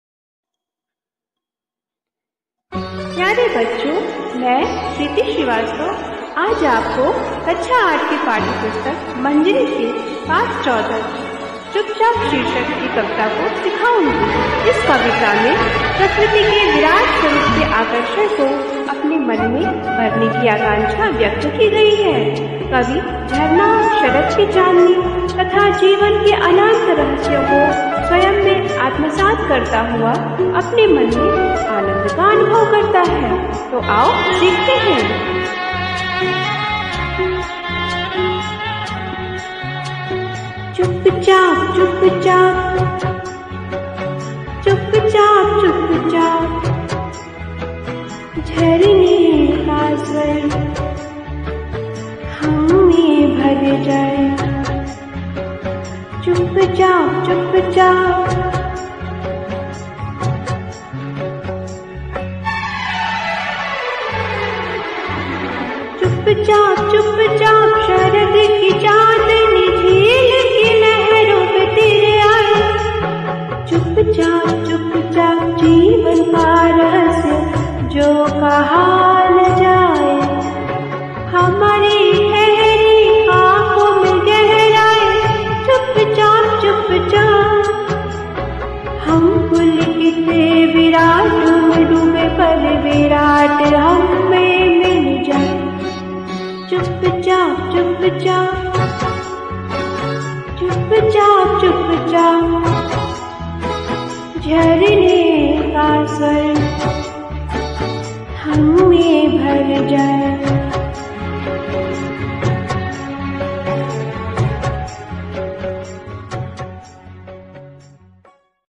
स्वरांजलि 70: चुपचाप (मंजरी-8)
बेसिक शिक्षा की कविताएं

मंजरी (हिंदी)
कक्षा -8
कविता - चुपचाप

चुप-चाप चुप-चाप
झरने का स्वर
हम में भर जाए,

चुप-चाप चुप-चाप
 शरद की चांदनी
झील की लहरों पर तिर आय,

चुप-चाप चुप-चाप
जीवन का रहस्य,

 जो कहा न जाए, हमारी
 ठहरी आंखों में गहराय,

चुपचाप चुपचाप
हम पुलकित विराट में डूबे
पर विराट हममे मिल जाए -

चुप-चाप चुप-चा$$प.

कवि: सच्चिदानंद हीरानंद वात्स्यायन अज्ञेय
शिक्षिका: प्रीति श्रीवास्तव
यूपीएस रन्नो, बक्शा
जौनपुर